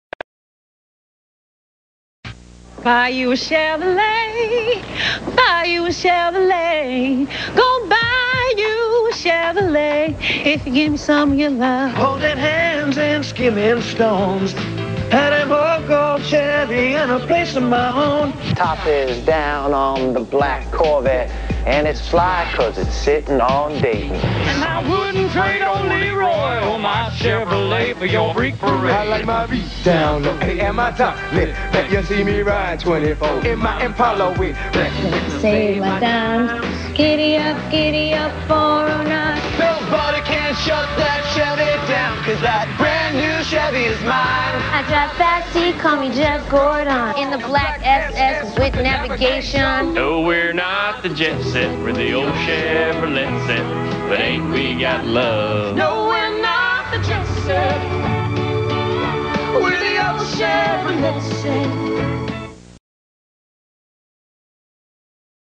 Chevy People Who Love Cars Love Chevy 2007
Że niby ludzie, którzy kochają auta, kochają Chevroleta. Reklama wrośnięta w kulturę amerykańską. Dla nas dość rażąca.